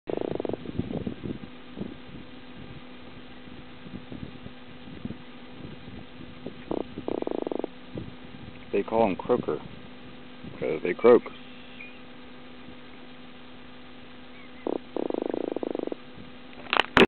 The Croaking Croaker
These were frequently hitting my baits and are fun to catch on light tackle. They make good bait for shark and larger trout.  A very sweet tasting fish too !